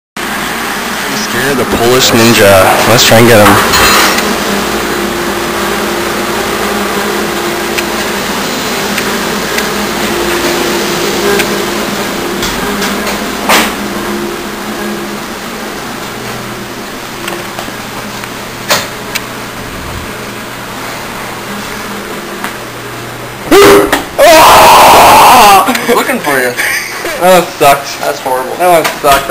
POLISH NINJA: HALLWAY "SCARE PRANK" FAIL #3
Another attempt to try and Scare the Ninja that Say's he can't be scared..We will get him one day.